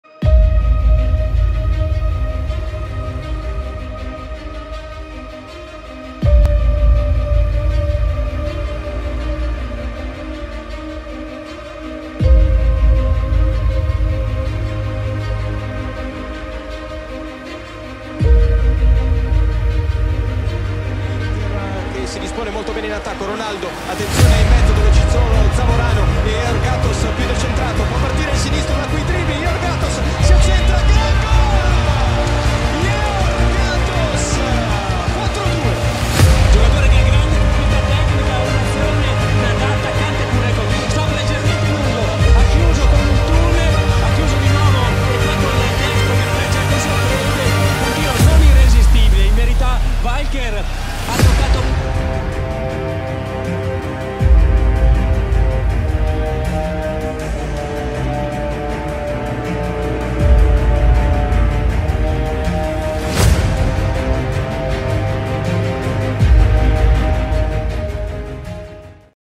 Georgatos Unbelievable Goal celebrating with ronaldo and zamorano
Grasshoppers vs. Inter (3:5)